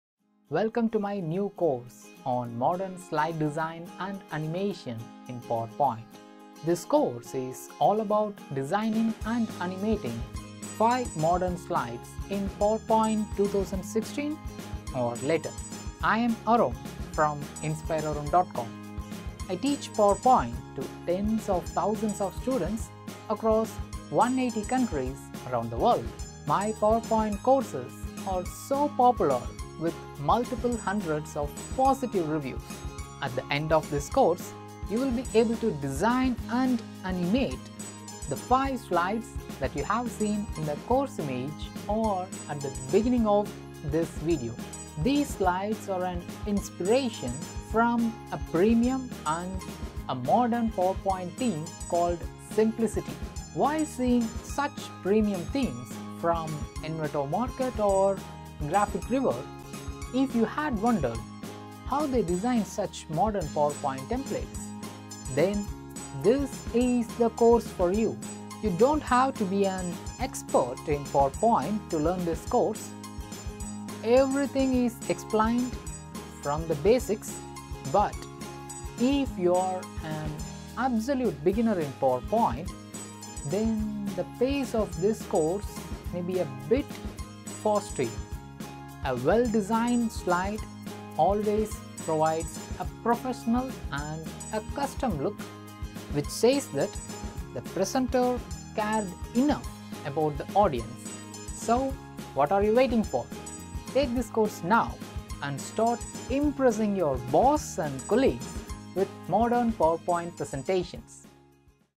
POWERPOINT PRESENTATION 2 - Modern Slide Design and Animation
You'll learn to Design 5 Modern, stunning slides from Simplicity Theme in PowerPoint. Add subtle, but modern animations to those modern slides you have designed in PowerPoint and equip yourself to design and animate PowerPoint presentations